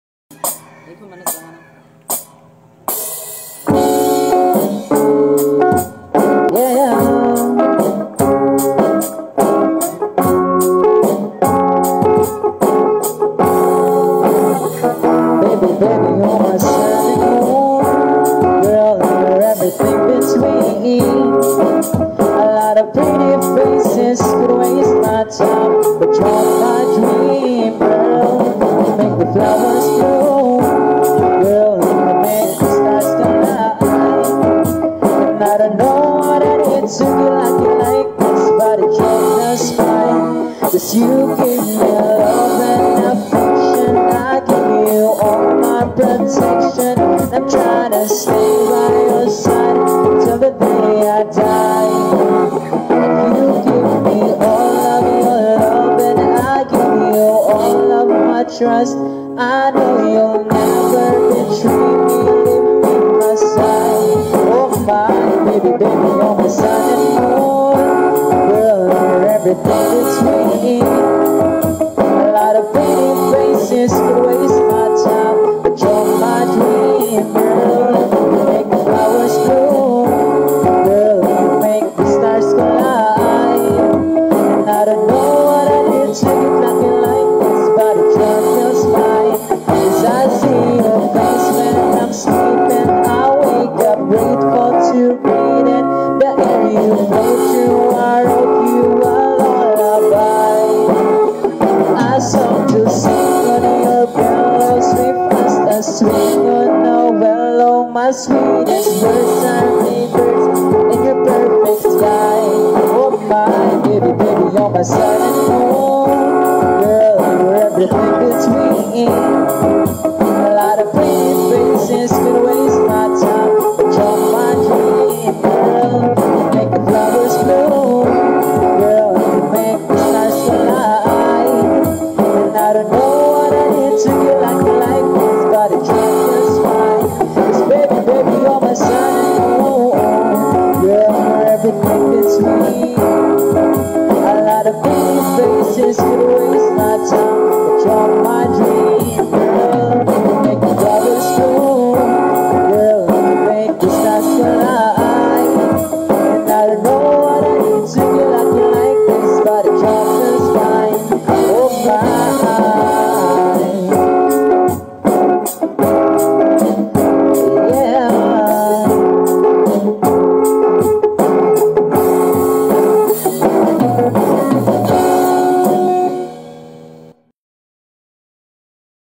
Acoustic Jam Session with Gozeph l Galing Kumanta
Sila ay magbarkada from Matalom,Leyte Philipppines.Yan po ang isa sa libangan nila pag walang trabaho o klase sila sa school.Galing nila di ba?